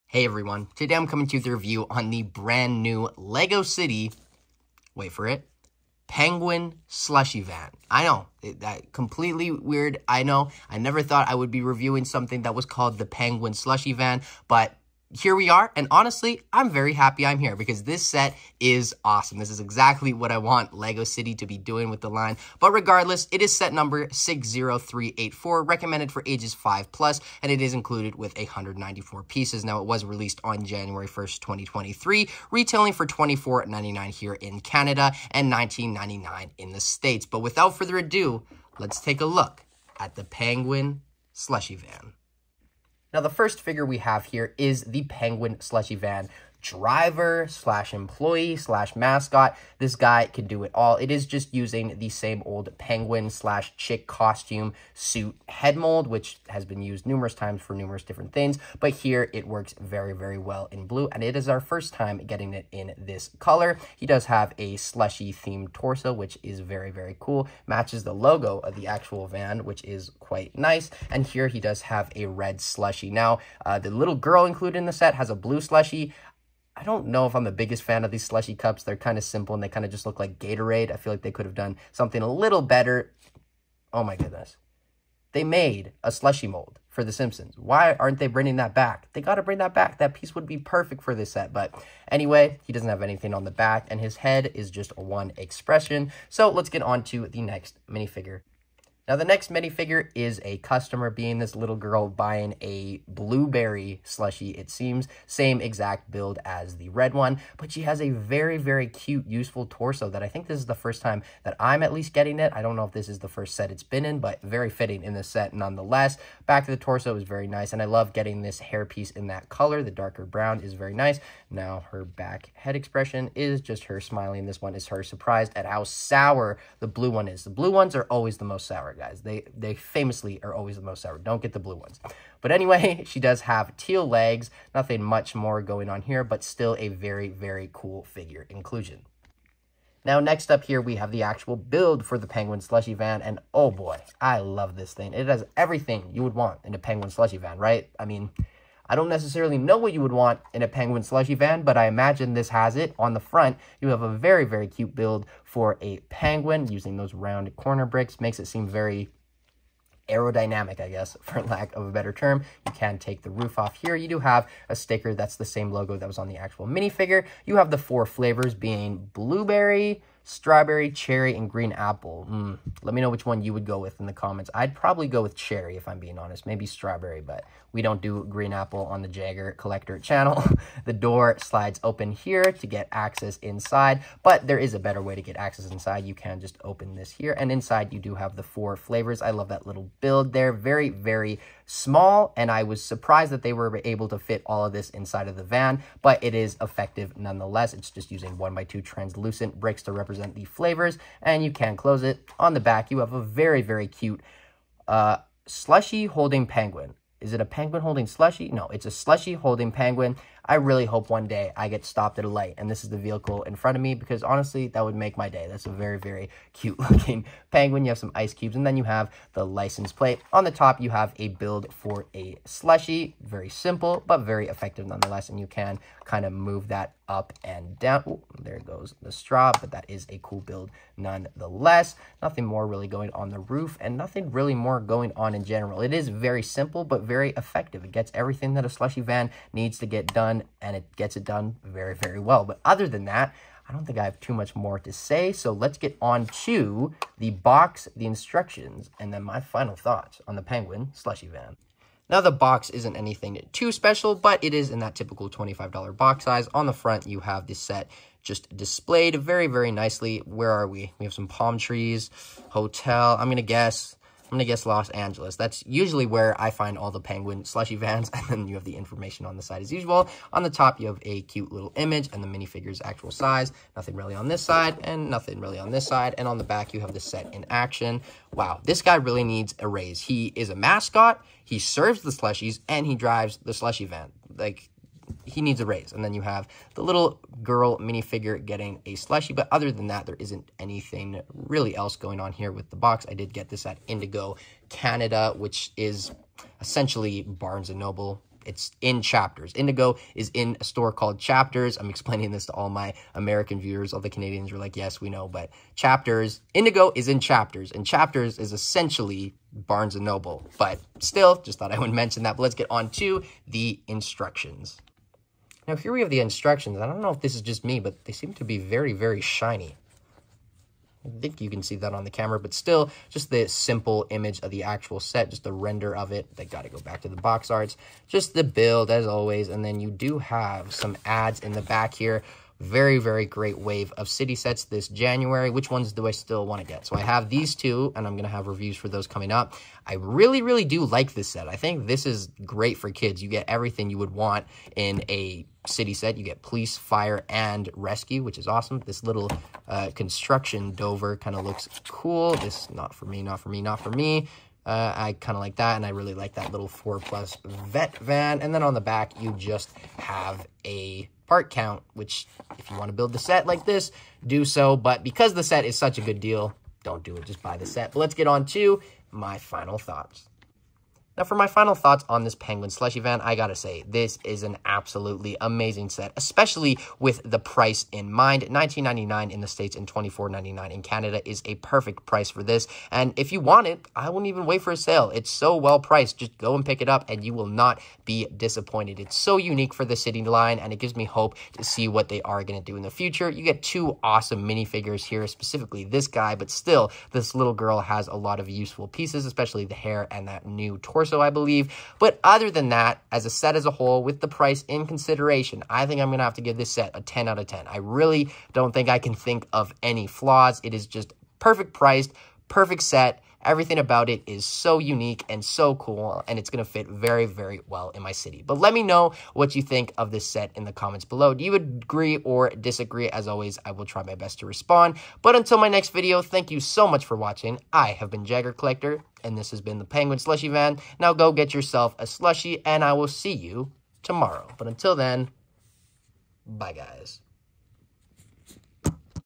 LEGO City Review: Set 60384 Penguin Slushy Van Review! (2023)
Today I will be reviewing the brand new Winter 2023 LEGO City Set 60384 Penguin Slushy Van!